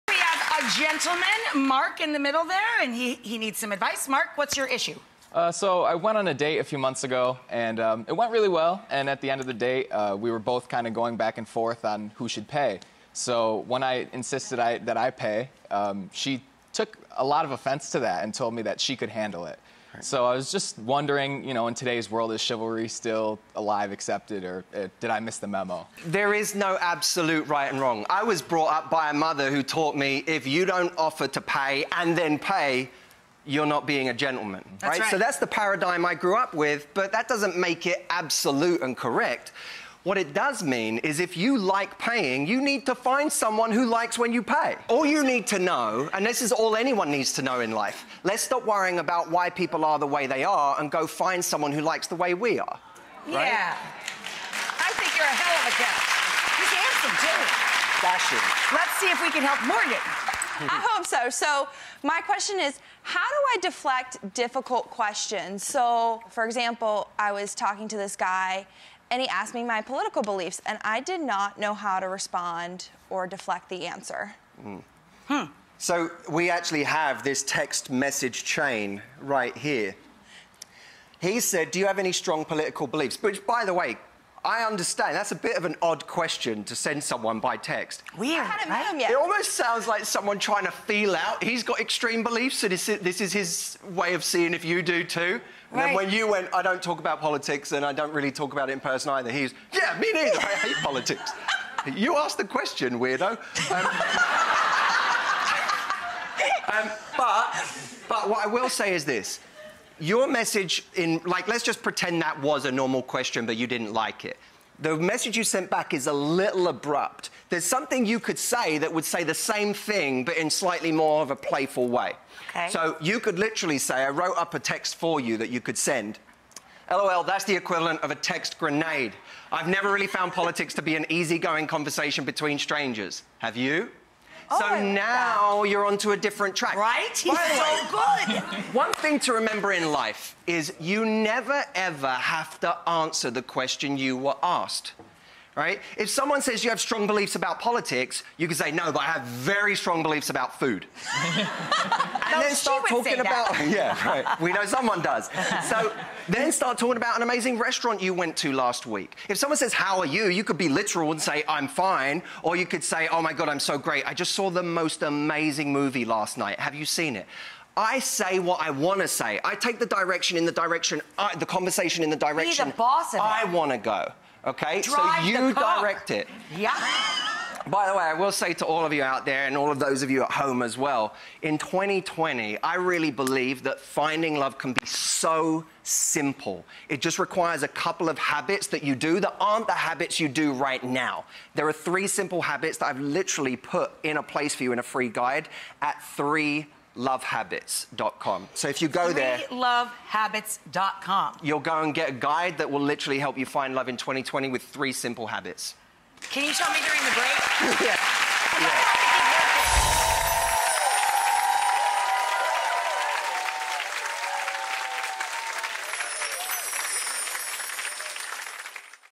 My Date Didn't Want Me To Pay For Her — What's The Right Move? | Dating Coach Matthew Hussey
Dating expert Matthew Hussey's advice for a guy who says his date got offended when he insisted on paying the bill makes SO much sense.